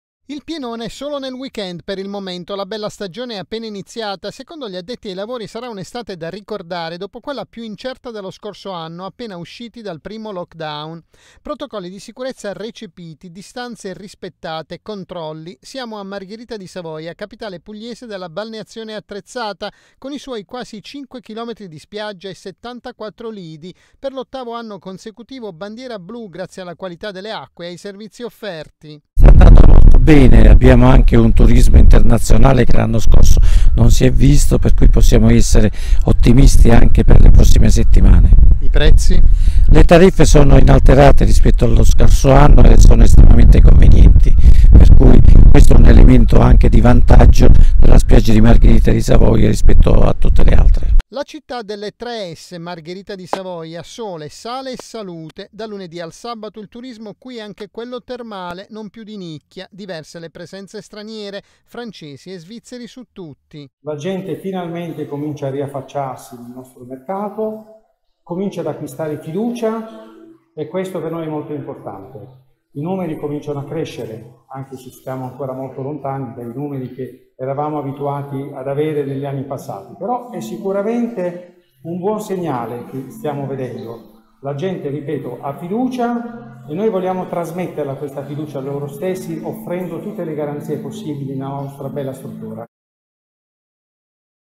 Margherita di Savoia, la stagione balneo termale
La stagione balneo termale sta entrando nel vivo. Si registra una buona partenza di turisti stranieri.